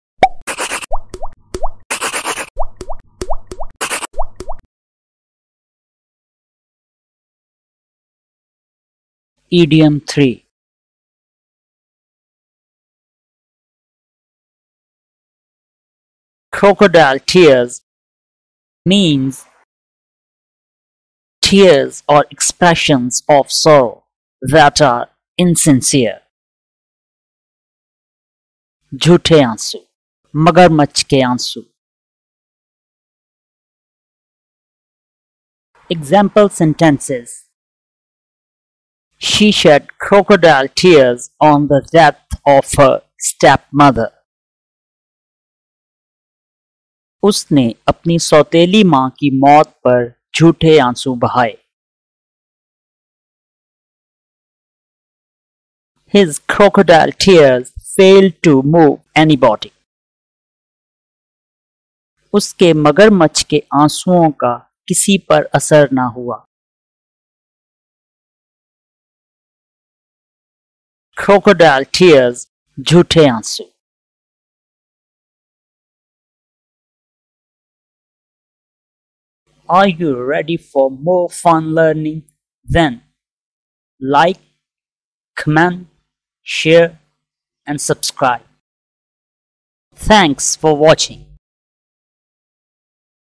Idiom 3 (Crocodile tears) - English Idioms & Phrases with Meanings and Examples in Urdu
Idioms are expressions that help us describe an exact situation in a different, more creative way. Understanding Idioms Can Boost Your Conversational English Skills. You may encounter idioms most often in spoken or written conversation. In this video, you will learn about idiom “Crocodile tears” meaning and sentences to understand it better.